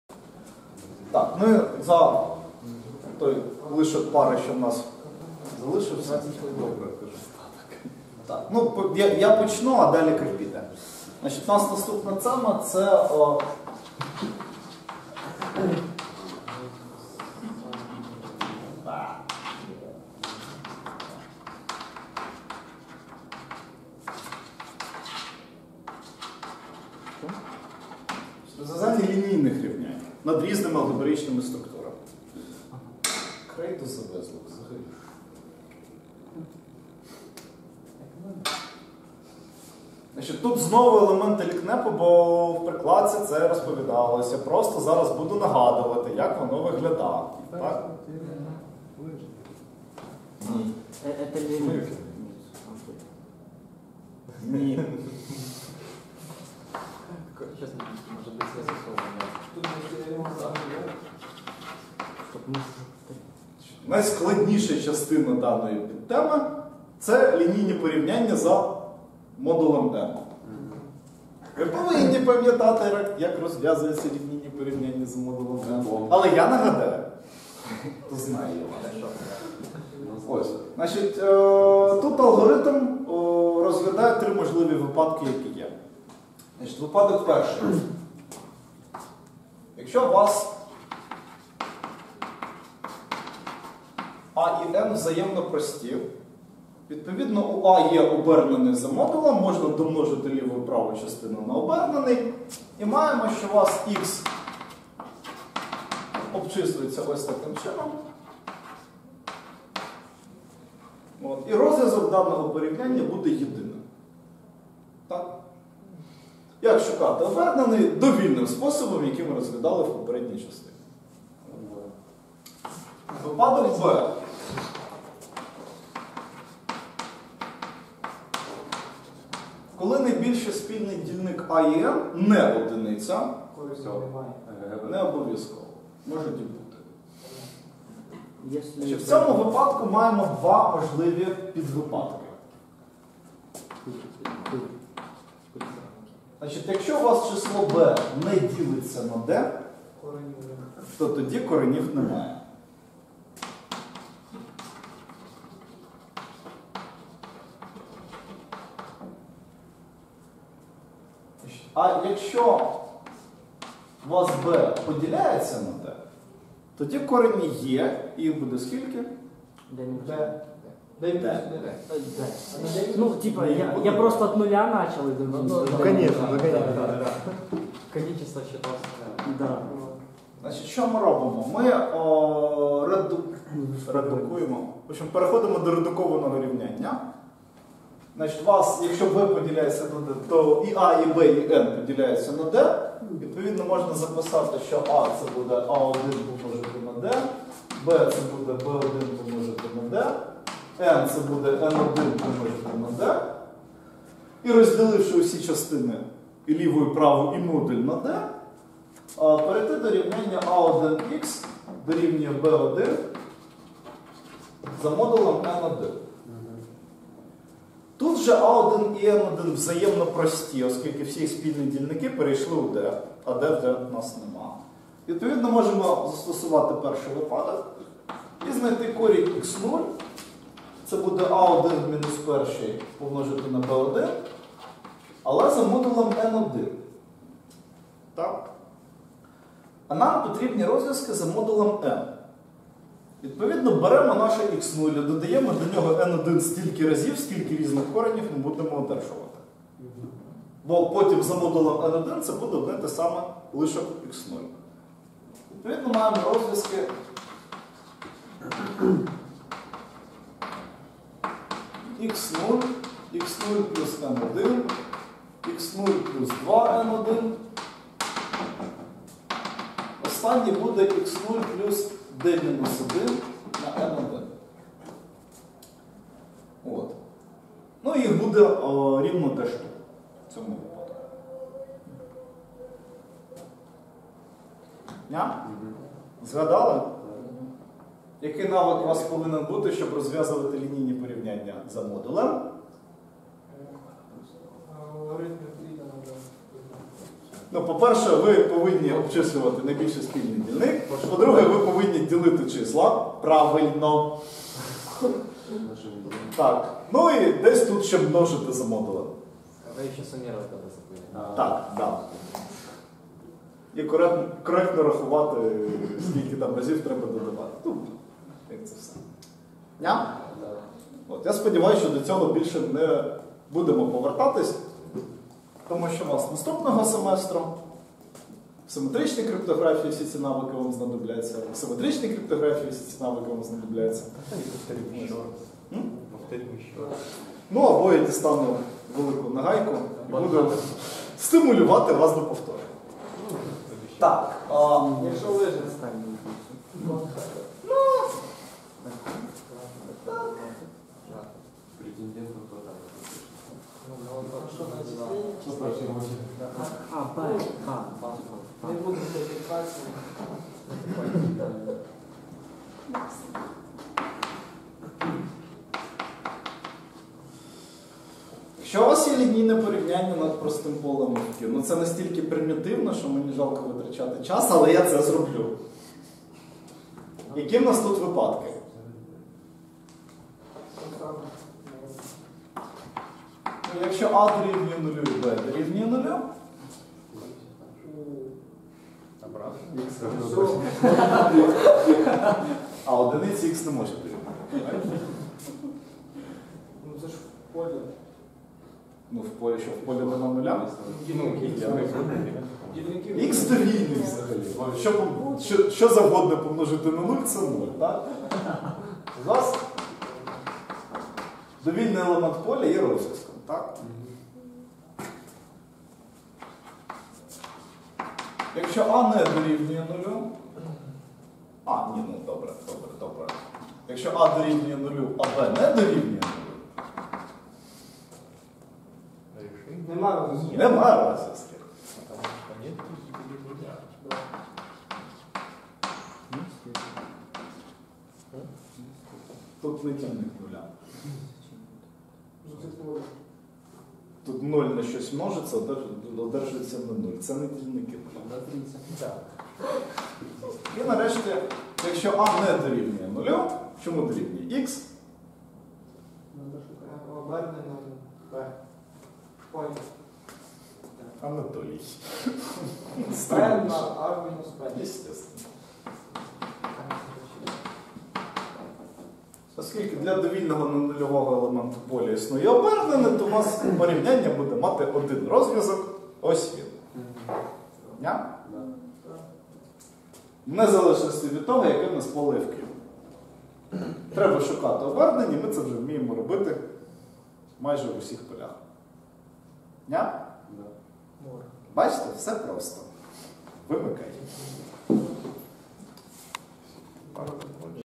СРОМ, лекція 05-3: розв'язання лінійних рівнянь у різних алгебраїчних системах
Курс "Спеціальні розділи обчислювальної математики", лекція №05
Розглянуто такі питання:
-- схема розв'язання лінійних порівнянь за модулем;
-- схема розв'язання лінійних рівнянь над скінченним полем.

Лектор: Сергій Яковлєв